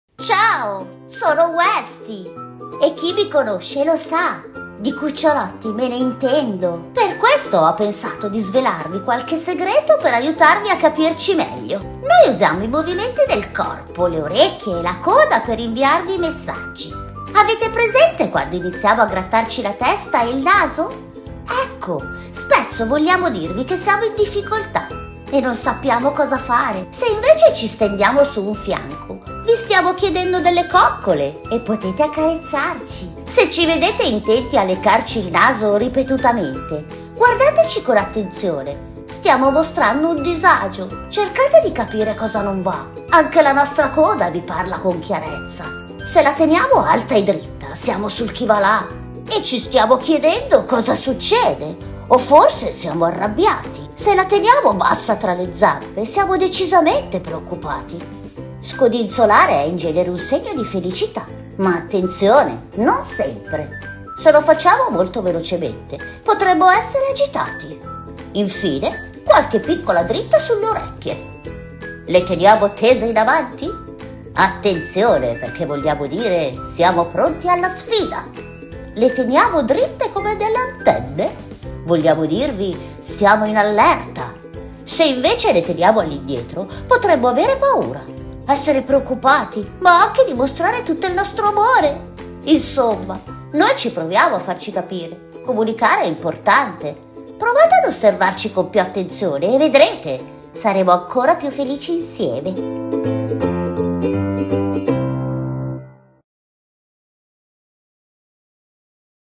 IL BLOG DI WESTIE
National Puppy Day, Enpa e Westie svelano il linguaggio dei nostri amici a quattro zampe.